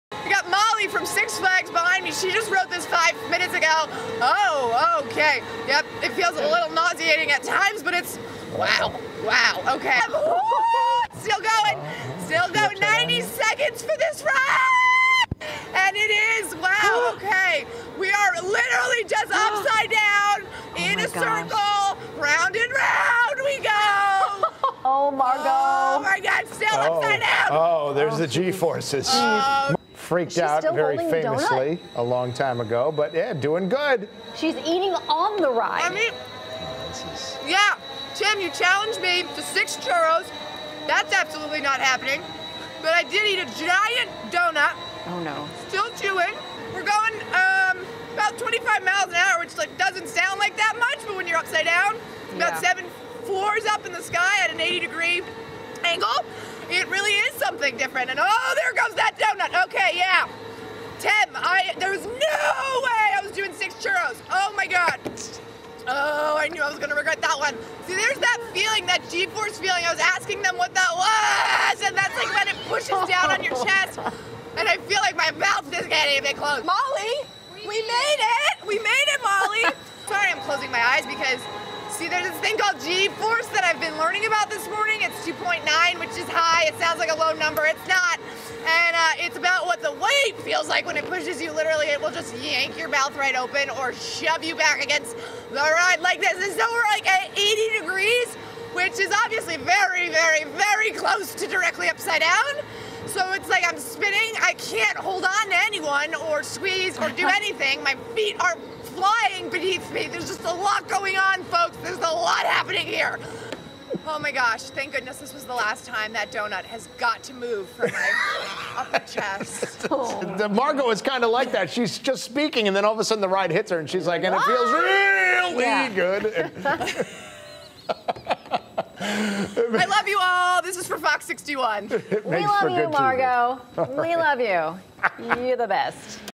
Six Flags New England: Supergirl Sky Flyer Highlights 2021
This is now my 4th year (well, 3 rides total bc of COVID) that I have been doing ride debuts at Six Flags New England. The first time I went, my first ride live segment got 1.7 million views. I've come a long way and feel much more composed reporting whilst riding a very intense attraction now (well, kinda)!